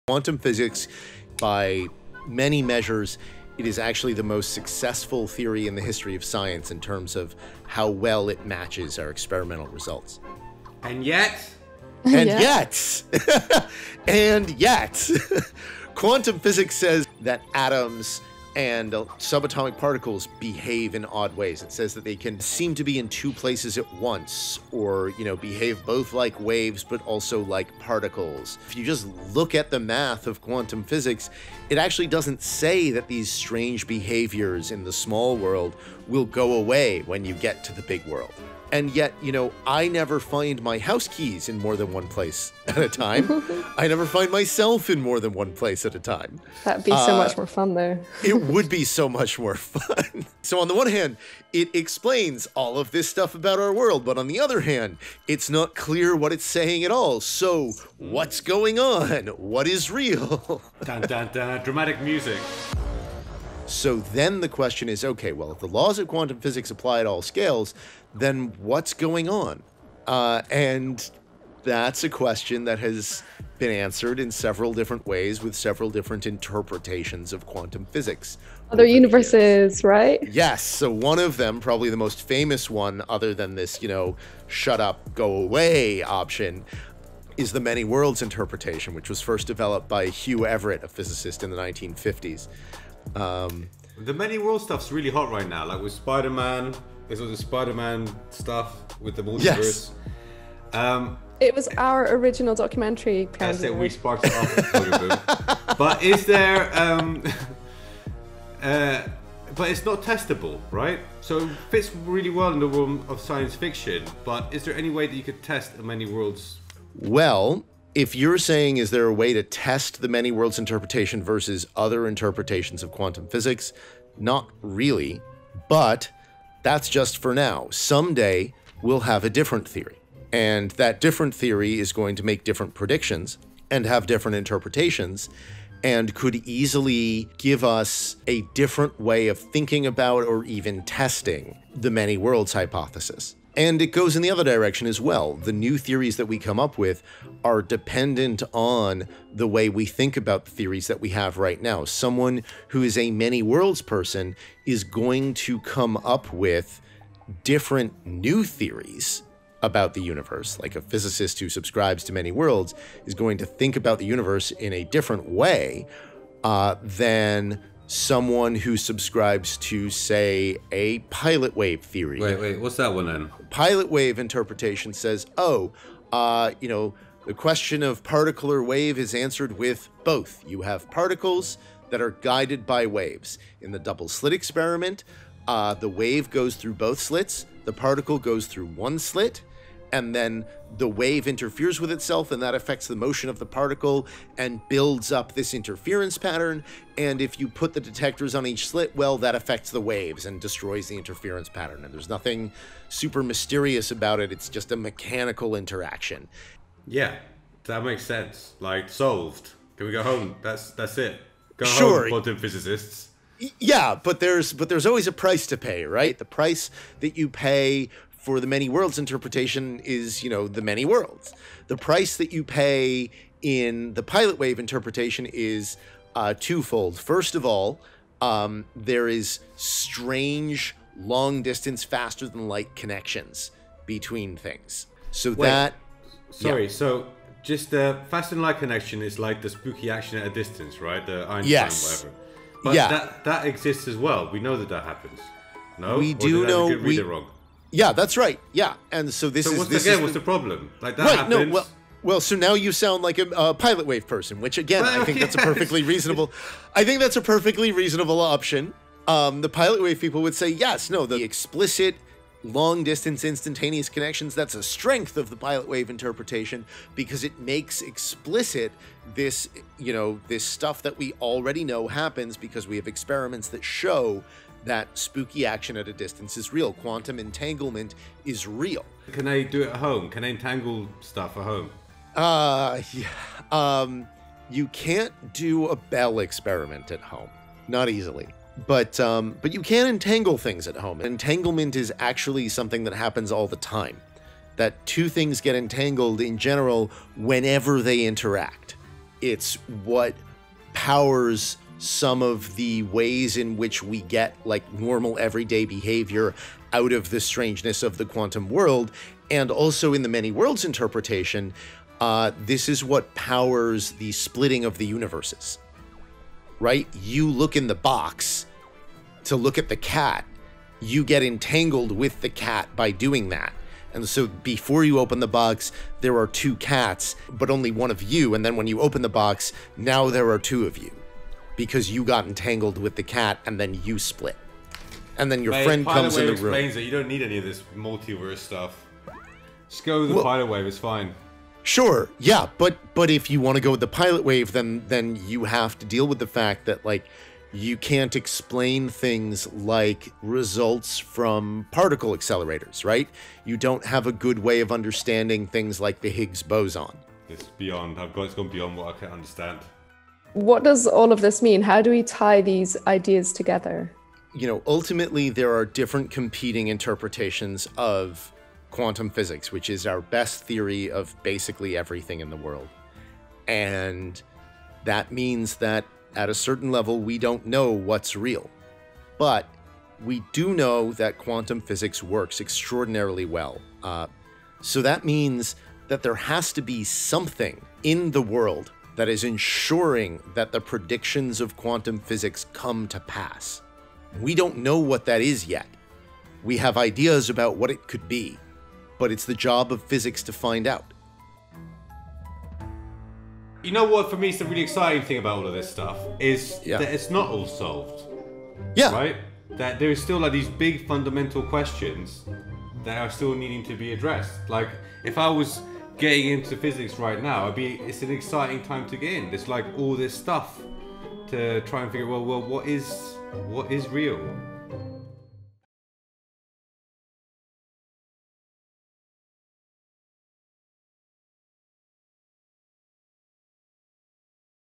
How do we know what's real? – BBC REEL
Quantum physics tells us that atoms and subatomic particles behave in strange ways. It says that they can be in two places at once and that they can behave both like waves but also like particles.

So how can we truly know what reality is?

A film by Pierangelo Pirak, Melissa Hogenboom and Adam Becker